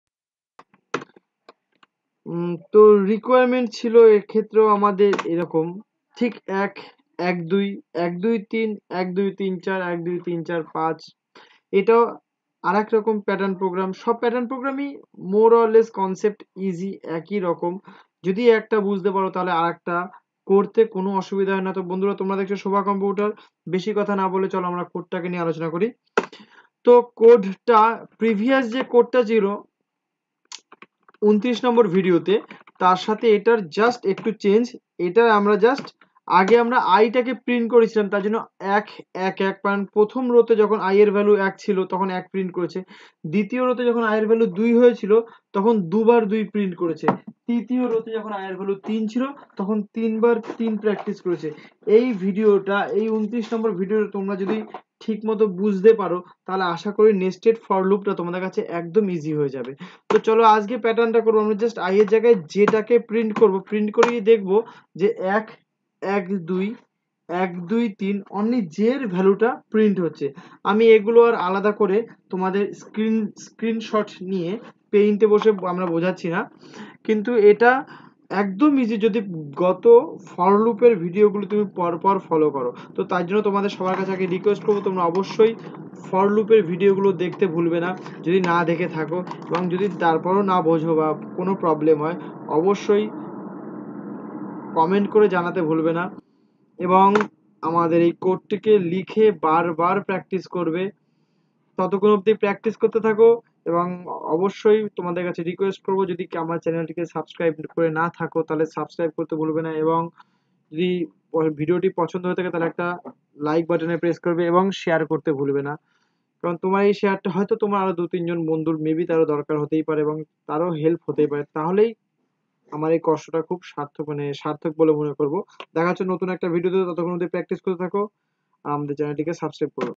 Nested For Loop Print J in C Programming Sova Academy
This Is another example of nested loop and we demonstrate a pattern program in C